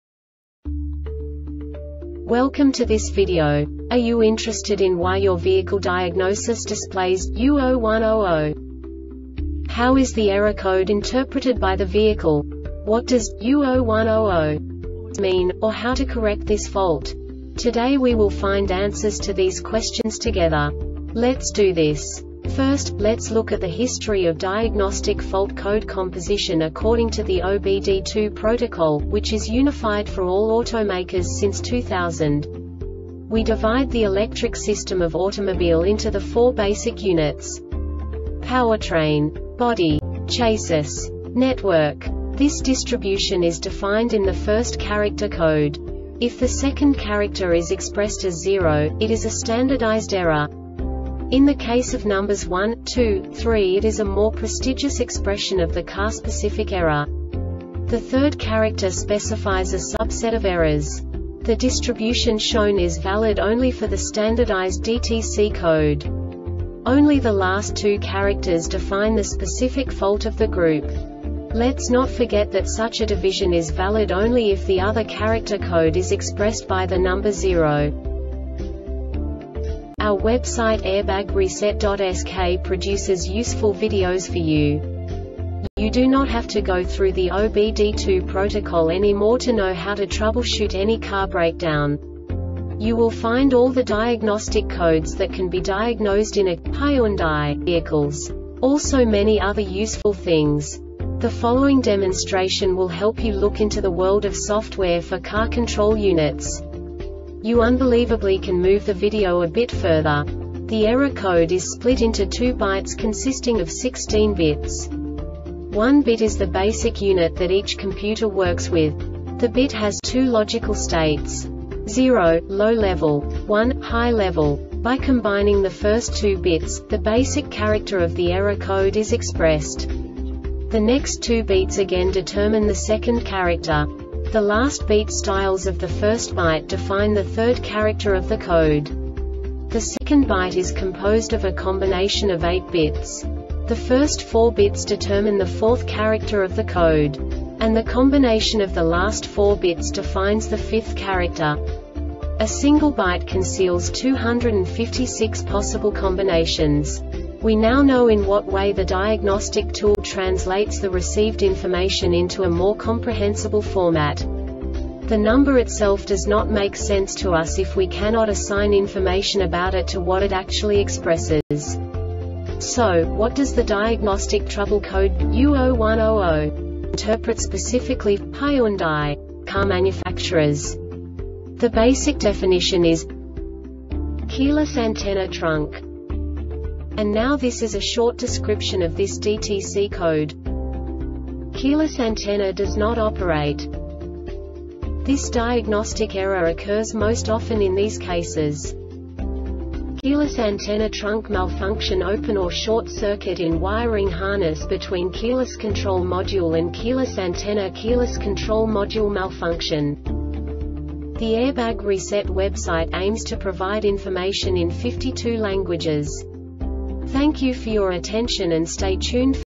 DTC Hyundai U0100 Short Explanation
Contents:
0:21 Basic DTC analysis according to OBD2 protocol standard.
1:48 Insight into programming
2:58 A diagnostically understandable description of the Diagnostic Trouble Code
3:05 Basic error definition

Make:
Hyundai
Code:
U0100
Definition:
Keyless Antenna (trunk)
Description:
Keyless antenna does not operate.
Cause:
Keyless antenna (trunk) malfunctionOpen or short circuit in wiring harness between keyless control module and keyless antennaKeyless control module malfunction